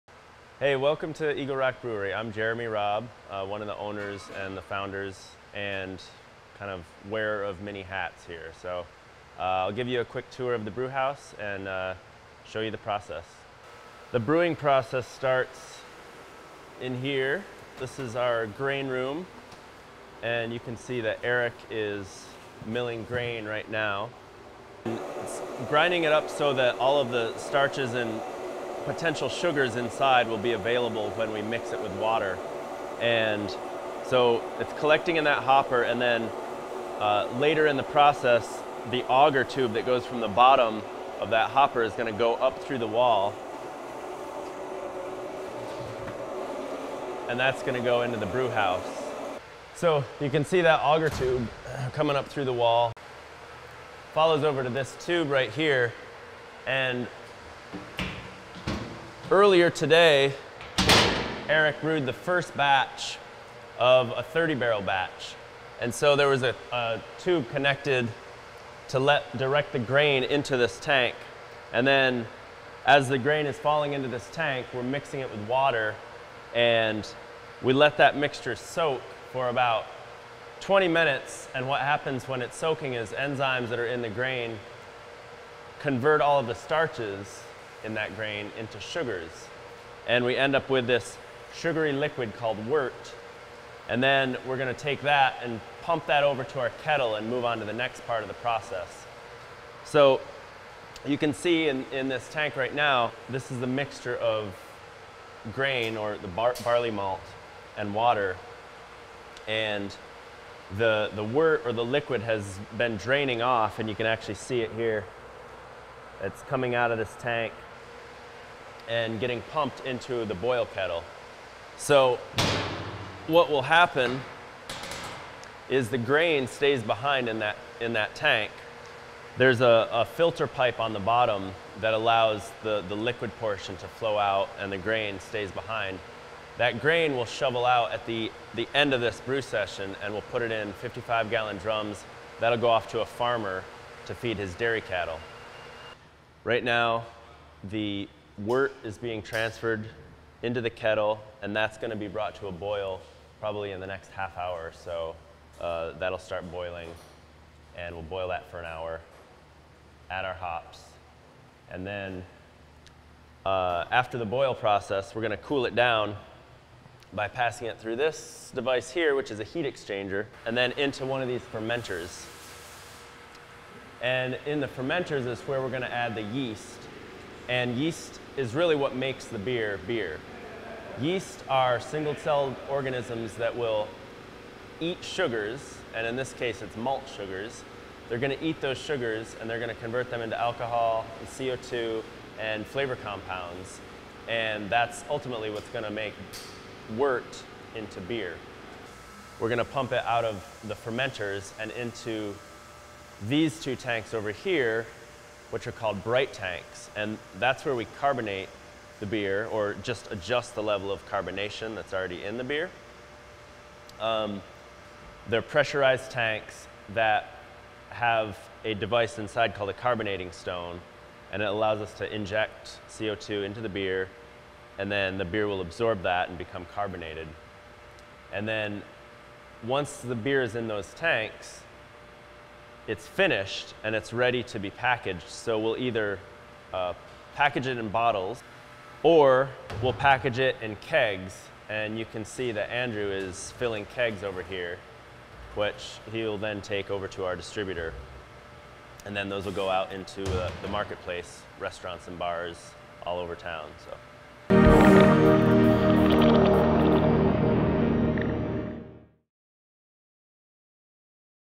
Eagle Rock Brewery Tour and Brewing Process
Join Thrash Lab on a tour of the Eagle Rock Brewery with owner, Jeremy Raub, who we interviewed for our episode of Subculture Club on craft beer Eagle Rock Brewery was the first operating brewery based in Los Angeles and for 60 years, they have been dedicated to the production of unique artisanal beers using the best ingredients to promote the ongoing development of the beer community.